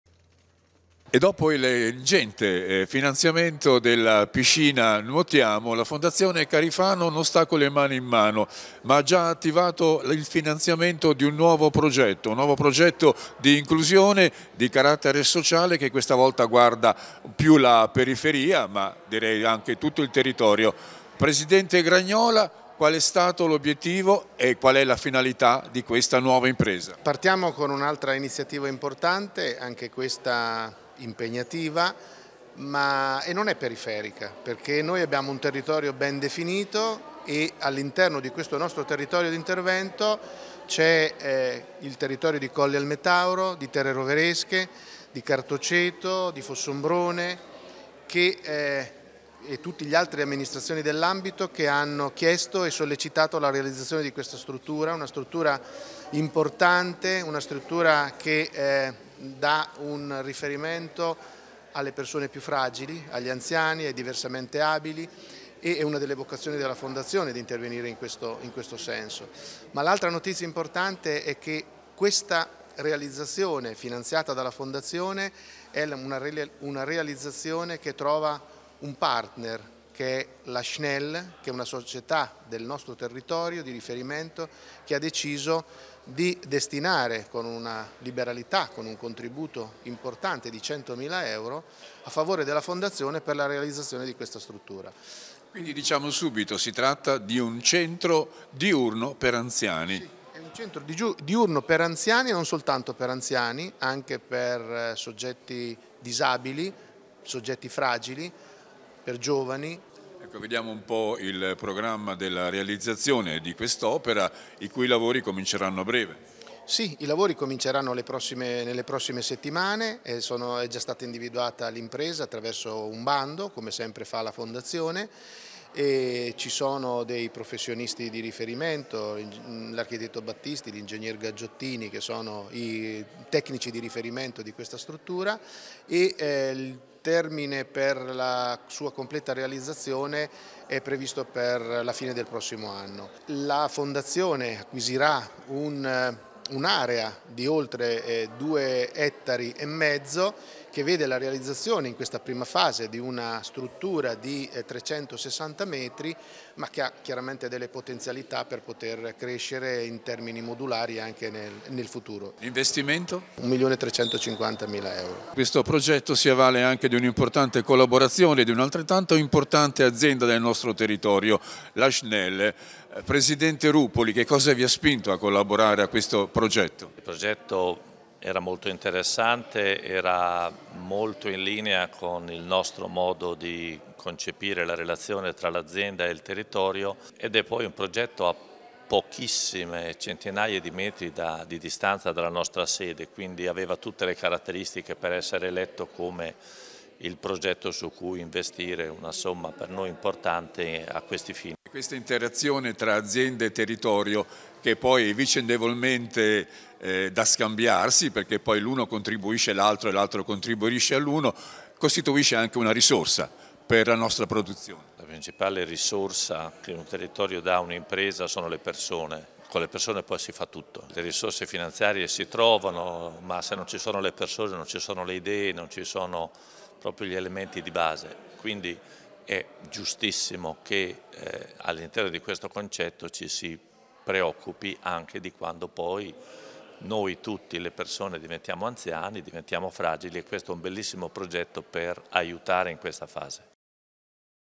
Un nuovo Centro Diurno a Colli al Metauro
Nuovo investimento della Fondazione Carifano. Avviato il progetto di un nuovo Centro Diurno per anziani e persone fragili a Colli al Metauro.